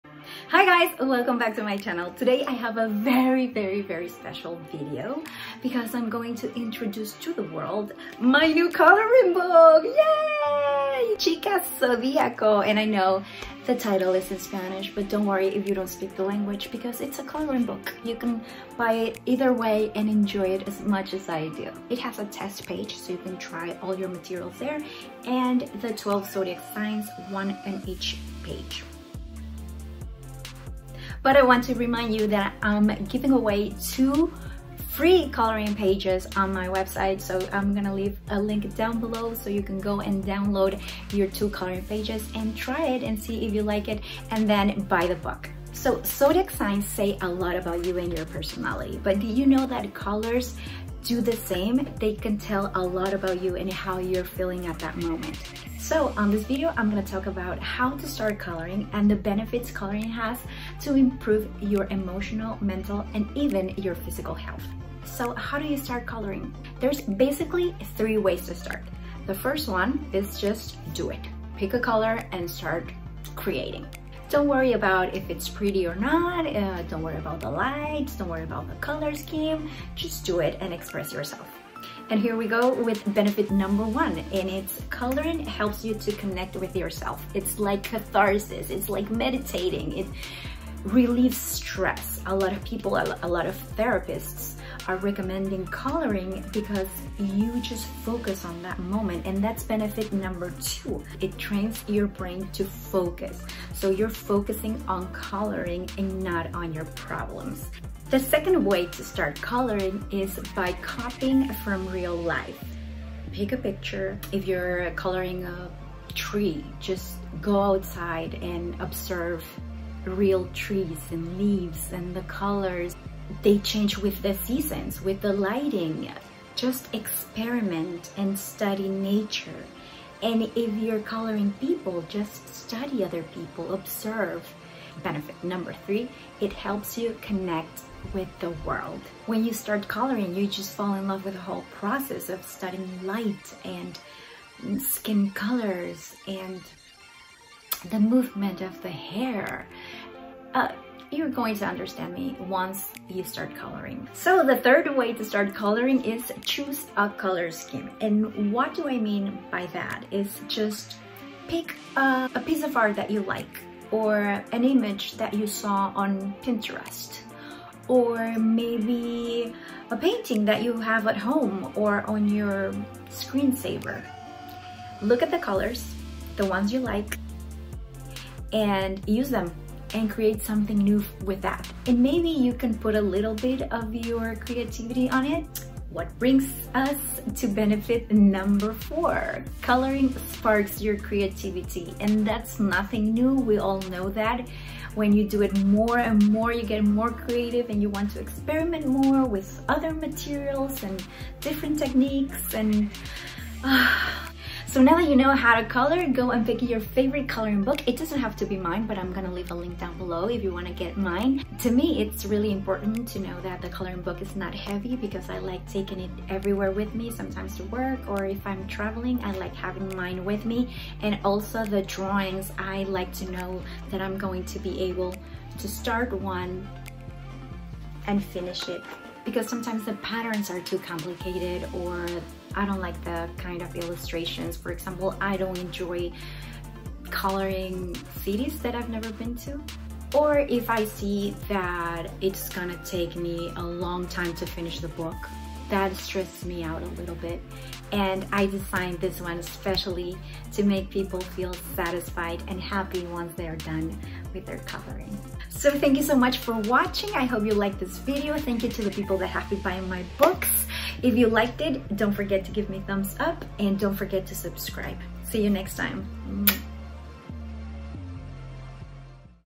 Coloring Books: 3 ways to start ❤ | SILVANA ARIAS
Basically, there are 3 ways to start coloring:
1. Just go for it and express yourself with any colors you want.
2. Copy from real life.
3. Choose a color scheme.

❤ Books /Libros: